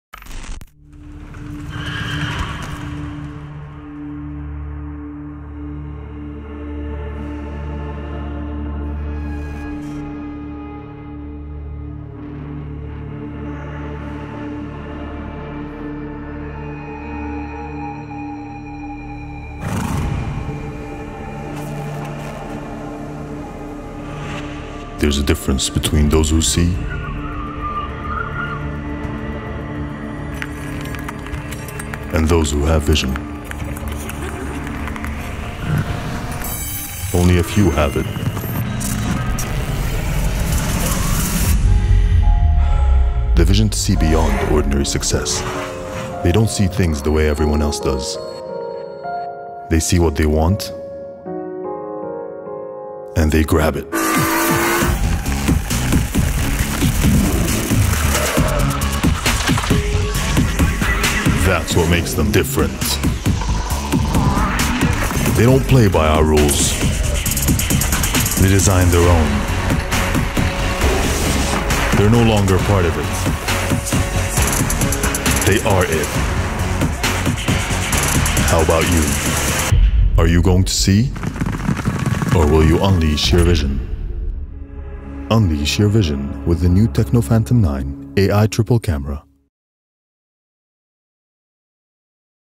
TECNO PHANTOM 9 Unleash Your Vision En
PHANTOM is back. This time, PHANTOM 9 redefines what is called masterpiece photography experience by its remarkable AI Triple Camera. With its constant pursuit and outstanding performance on camera, PHANTOM 9 helps you record those unforgettable moments as well as satisfy your interest of taking photos. Liberate your thought, broaden your view, get more of what you love.